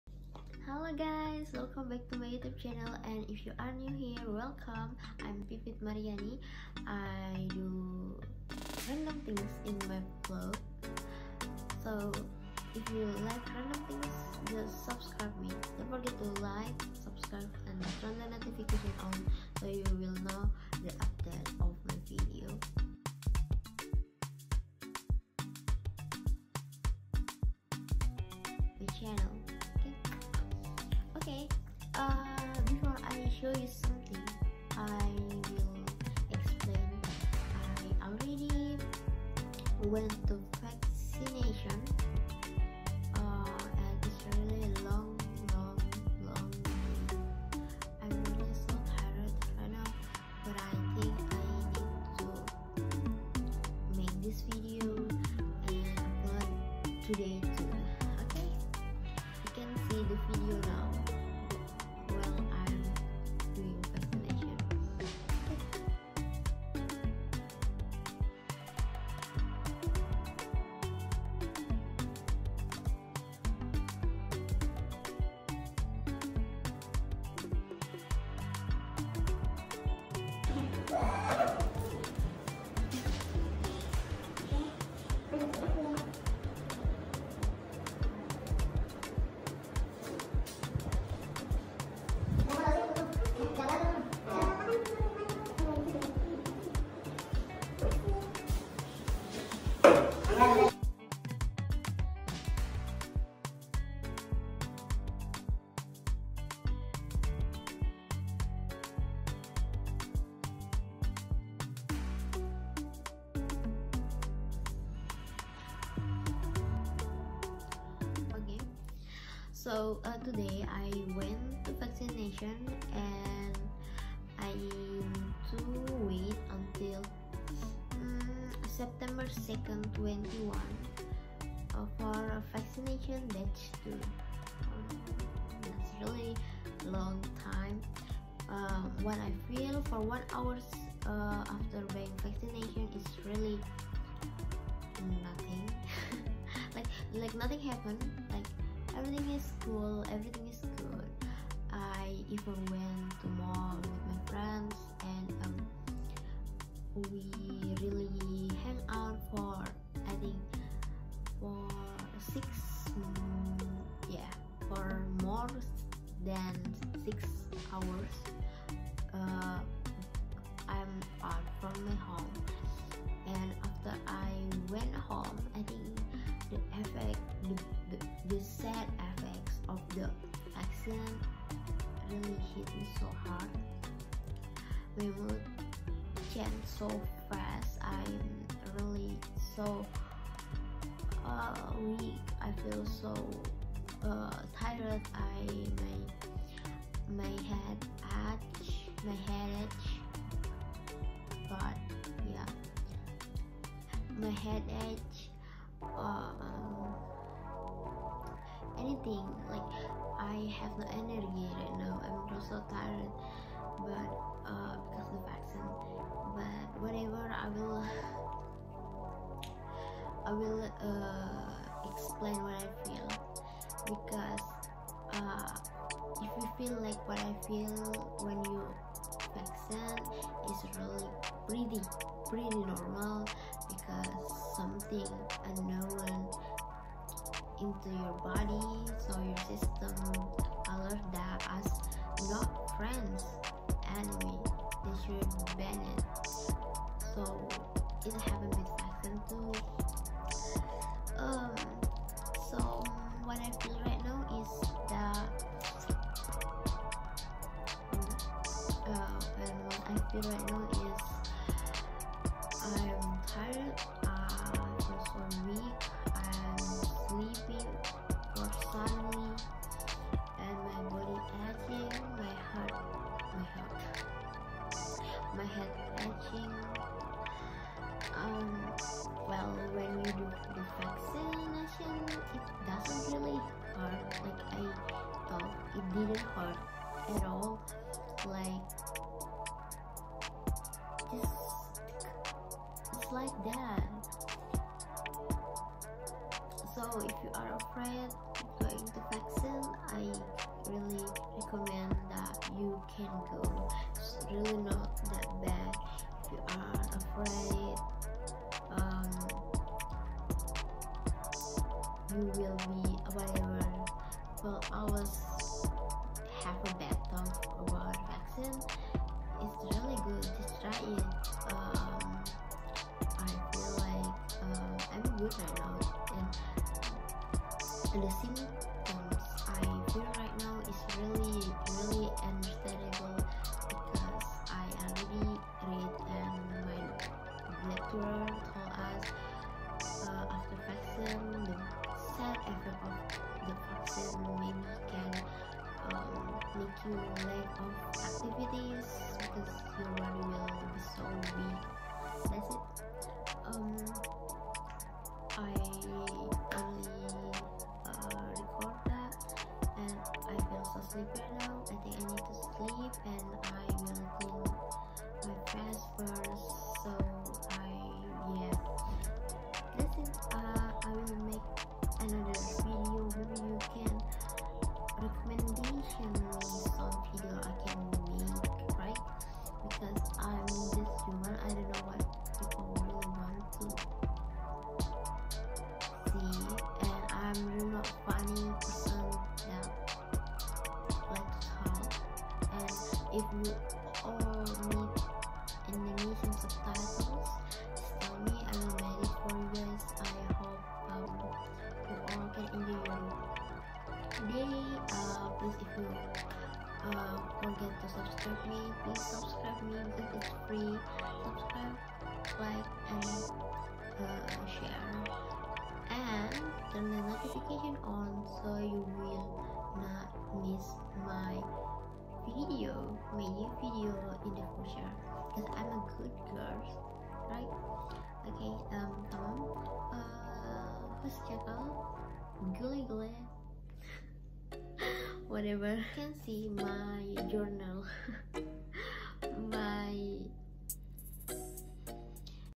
The experience when I did vaccination //
Hello, it's my first-time vaccination so I want to make memories about it. Also, I just explained what I feel after vaccination. If there's any question, don't be shy to ask me.

Recorded by iPhone Xr
Edited by VLLO and iNshot